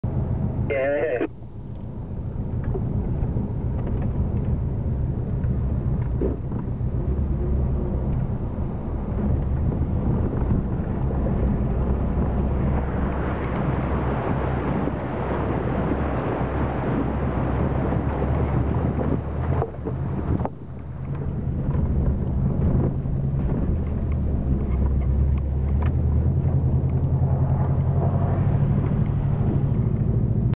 Urbenville trip 2/1/2011
This is our trip to Urbenville we are crossing the Clarence River at Paddys flat the river was a little high.